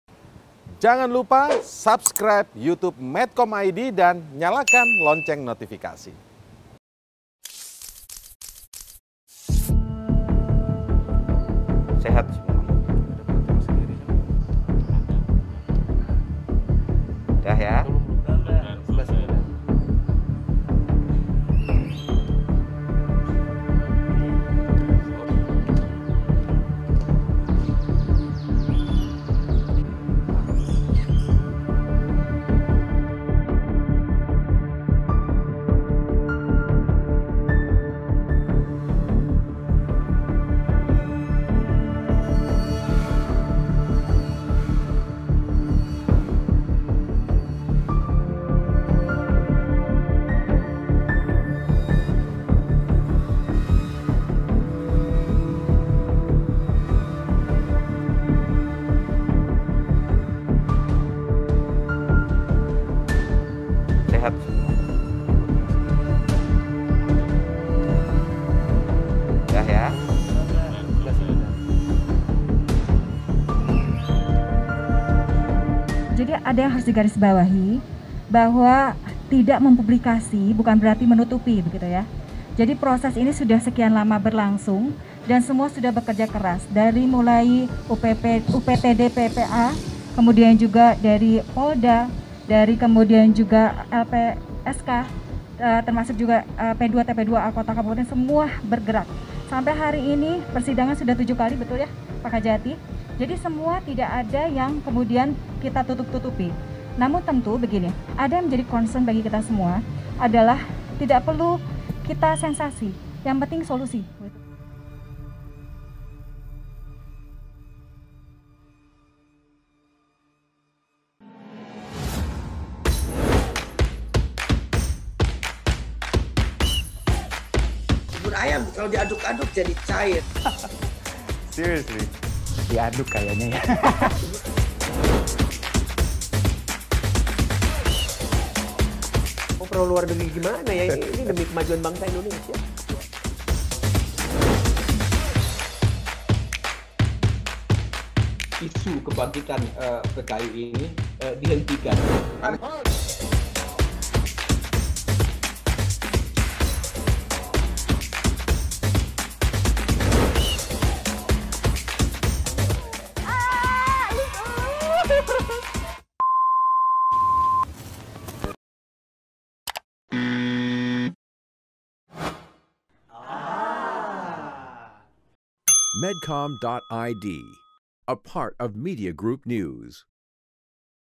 Istri Ridwan Kamil Bantah Pemerintah Tutupi Kasus Pemerkosaan 12 Santriwati
Istri Gubernur Jawa Barat, Atalia Praratya Kamil membantah tudingan dari sejumlah pihak bahwa pemerintah menutupi kasus pemerkosaan 12 santriwati. Menurut Atalia, upaya cepat perlindungan korban dan. penegakan hukum terhadap pelaku lebih penting dari pada publikasi

Atalia menegaskan kasus pemerkosaan itu tidak dipublikasi untuk menghormati hak asasi dan proses pemulihan korban. Ia juga memastikan proses hukum sudah berjalan sejak enam bulan lalu. Atalia menambahkan sejak kasus terkuak pada Mei 2021, pemerintah dan penegak hukum langsung turun tangan dalam menyelesaikan kasus tersebut.